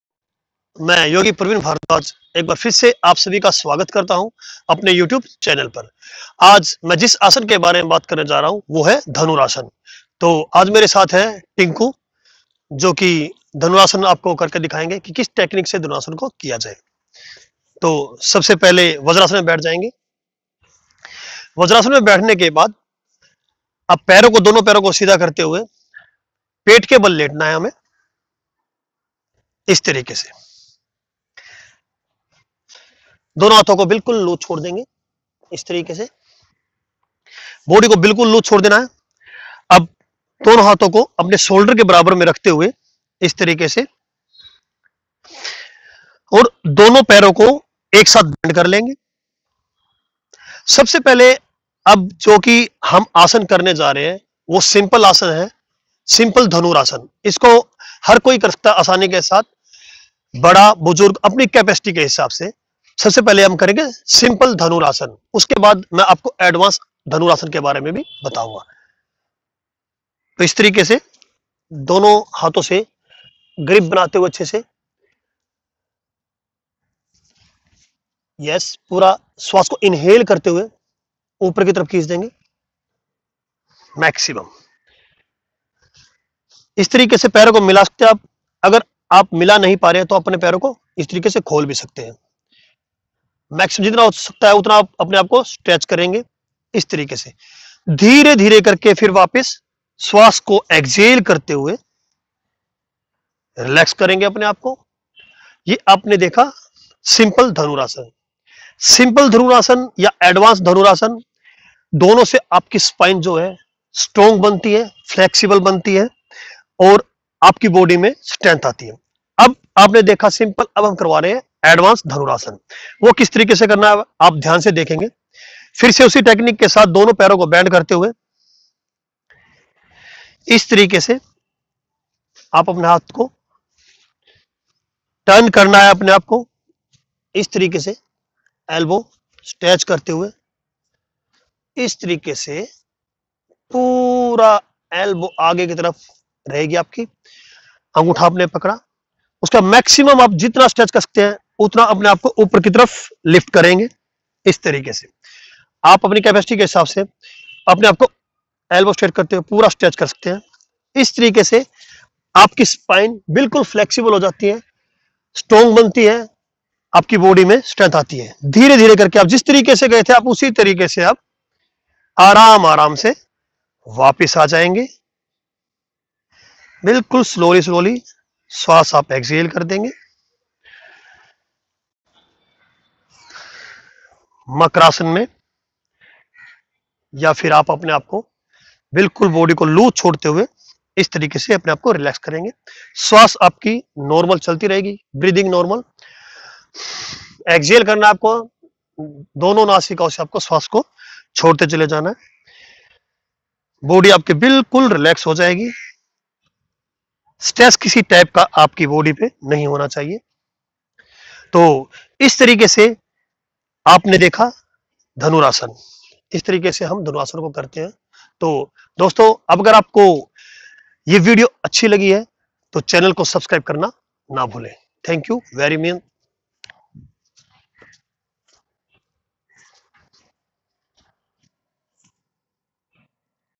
Dhanurasana will make your spine strong
Dhanurasana will make your spine strong n reduce your tummy,yogaforhealth yogaforall fat.